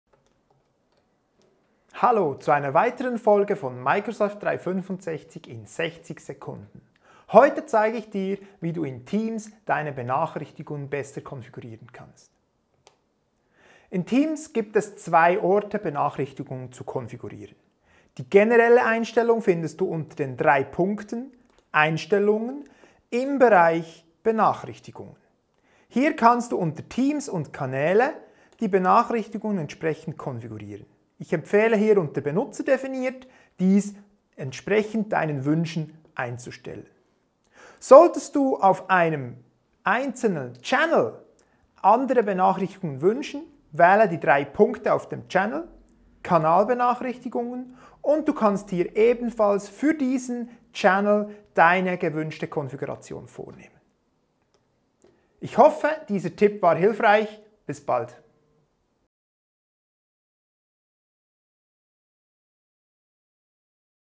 Microsoft 365 in 60 Sekunden - Benachrichtigungen in Teams konfigurieren
Hello hello, willkommen zu einer weiteren Folge von Microsoft 365 in 60 Sekunden. Heute zeigt Andreas dir, wie du deine Benachrichtigungen in Microsoft Teams besser konfigurieren kannst.
Dabei geht er auch darauf ein, wie du für einzelne Channel gewünschte Konfigurationen einstellen kannst.

Hast du Themenwünsche für ein nächstes Microsoft 365 in 60 Sekunden Video? Cool, dann lass doch einfach ein Kommentar da. Wir freuen uns!

Besuche auch unsere Website für mehr Inhalte rundum das Thema Microsoft 365